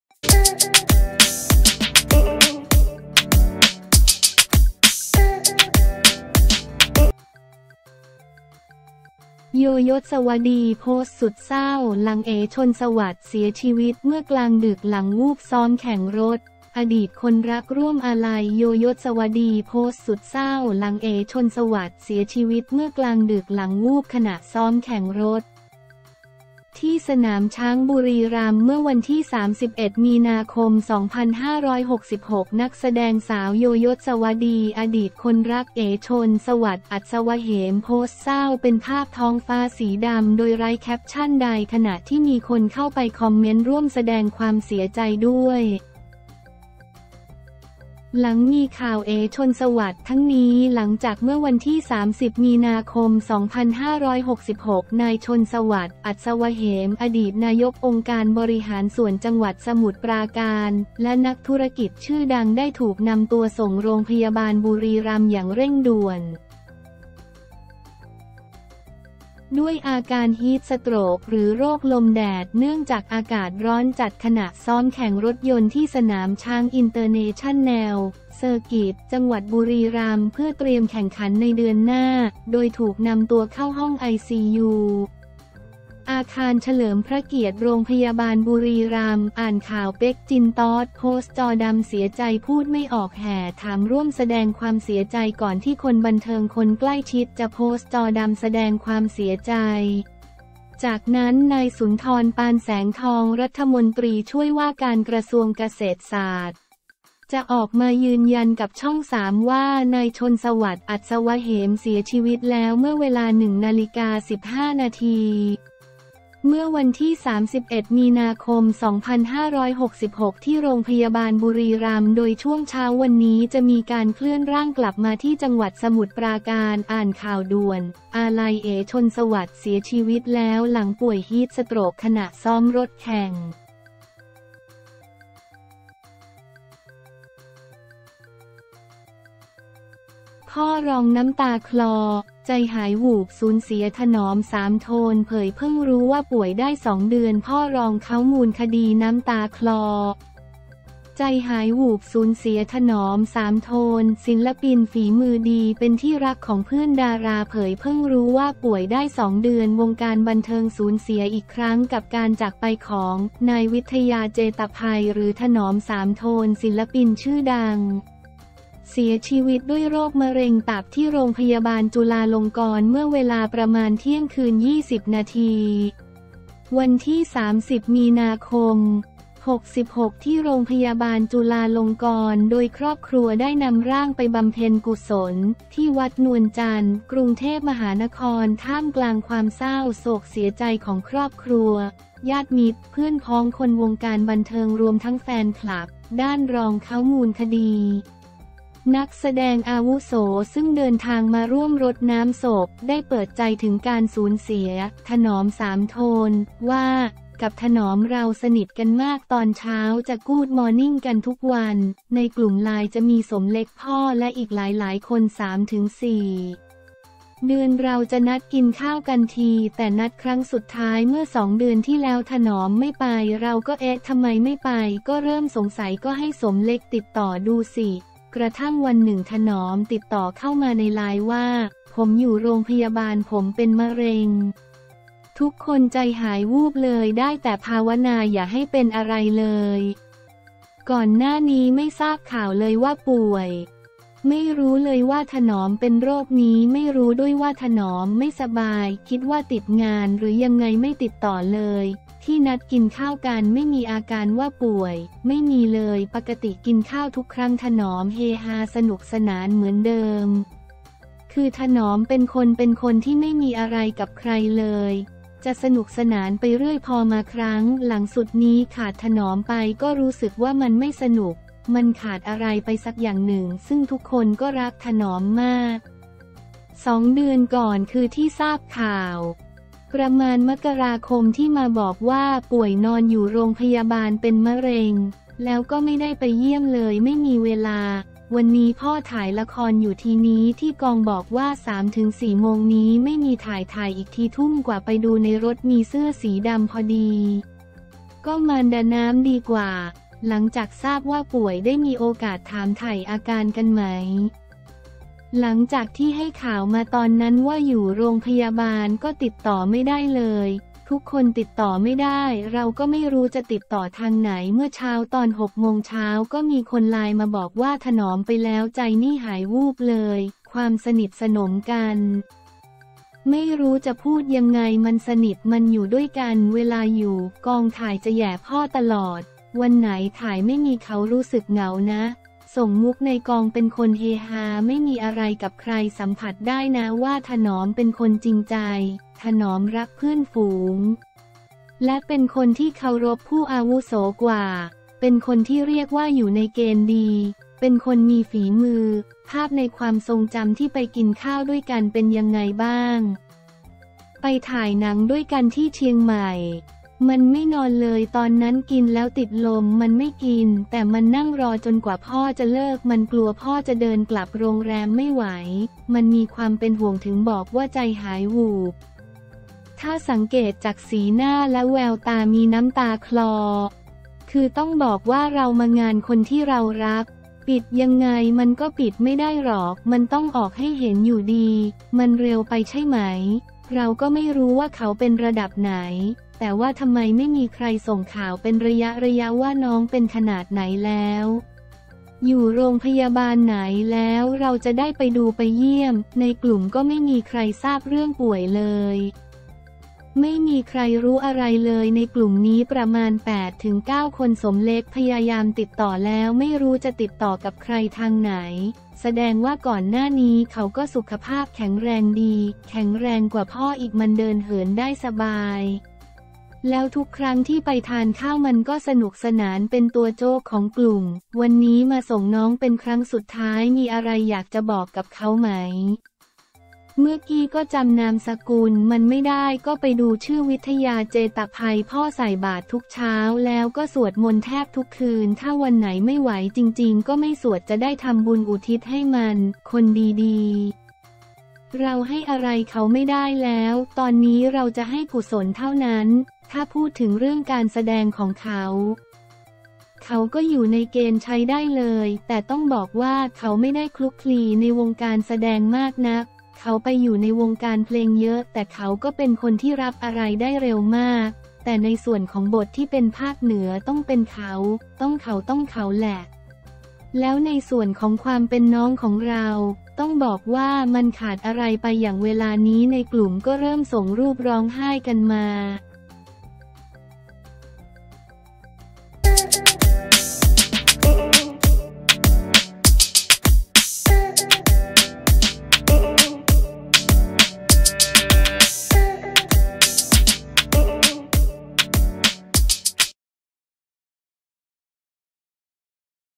โย ยศวดี โพสต์สุดเศร้า หลัง เอ๋ ชนม์สวัสดิ์ เสียชีวิตเมื่อกลางดึกหลังวูบซ้อมแข่งรถ
อดีตคนรักร่วมอาลัย โย ยศวดี โพสต์สุดเศร้า หลัง เอ๋ ชนม์สวัสดิ์ เสียชีวิตเมื่อกลางดึก หลังวูบขณะซ้อมแข่งรถ ที่สนามช้างฯ บุรีรัมย์

เมื่อวันที่ 31 มีนาคม 2566 นักแสดงสาว โย ยศวดี อดีตคนรัก เอ๋ ชนสวัสดิ์ อัศวเหม โพสต์เศร้าเป็นภาพท้องฟ้าสีดำ โดยไร้แคปชั่นใด ขณะที่มีคนเข้าไปคอมเมนต์ ร่วมแสดงความเสียใจด้วย หลังมีข่าว เอ๋ ชนม์สวัสดิ์

ทั้งนี้หลังจากเมื่อวันที่ 30 มีนาคม 2566 นายชนม์สวัสดิ์ อัศวเหม อดีตนายกองค์การบริหารส่วนจังหวัดสมุทรปราการ และนักธุรกิจชื่อดัง ได้ถูกนำตัวส่ง รพ.บุรีรัมย์อย่างเร่งด่วน ด้วยอาการฮีทสโตรก หรือโรคลมแดด เนื่องจากอากาศร้อนจัด ขณะซ้อมแข่งรถยนต์ที่สนามช้างอินเตอร์เนชั่นแนล เซอร์กิต จังหวัดบุรีรัมย์ เพื่อเตรียมแข่งขันในเดือนหน้า โดยถูกนำตัวเข้าห้องไอซียู อาคารเฉลิมพระเกียรติ รพ.บุรีรัมย์
อ่านข่าว – เป๊ก-จิน-ต๊อด โพสต์จอดำ เสียใจพูดไม่ออก แห่ถาม-ร่วมแสดงความเสียใจ
ก่อนที่คนบันเทิงคนใกล้ชิดจะโพสต์จอดำแสดงความเสียใจ จากนั้น นายสุนทร ปานแสงทอง รมช.เกษตรฯ จะออกมายืนยันกับช่อง3 ว่า นายชนม์สวัสดิ์ อัศวเหม เสียชีวิตแล้วเมื่อเวลา 01.15 น. เมื่อวันที่ 31 มีนาคม 2566 ที่โรงพยาบาลบุรีรัมย์ โดยช่วงเช้าวันนี้จะมีการเคลื่อนร่างกลับมาที่ จ.สมุทรปราการ
อ่านข่าว – ด่วน! อาลัย เอ๋ ชนม์สวัสดิ์ เสียชีวิตแล้ว หลังป่วยฮีทสโตรก ขณะซ้อมรถแข่ง